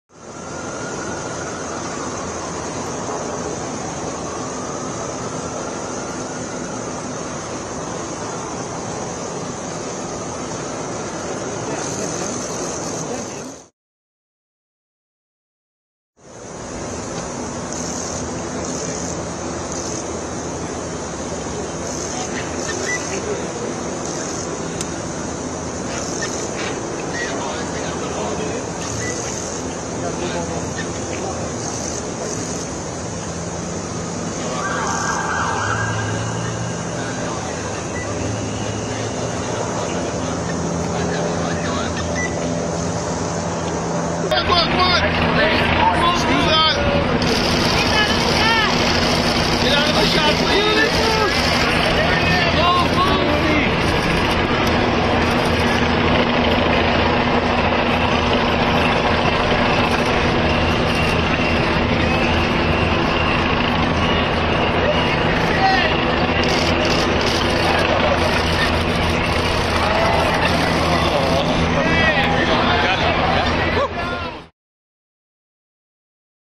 Trump arrives at NYC courthouse for arraignment
Donald Trump, the former U.S. president and front-runner for the 2024 Republican nomination, arrived Tuesday (April 4) at a New York City courthouse to be arraigned on charges related to an alleged hush-money payment to porn star Stormy Daniels.